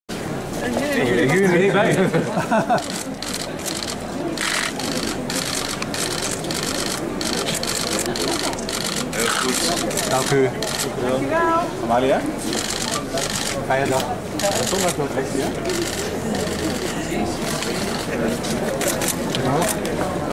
King Willem-Alexander, Queen Máxima of the Netherlands with their daughters
King Willem-Alexander, queen Máxima, princesses Amalia, Alexia and Ariane attending the baptism of Carlos, prince of Piacenza, in Parma. september 25th 2016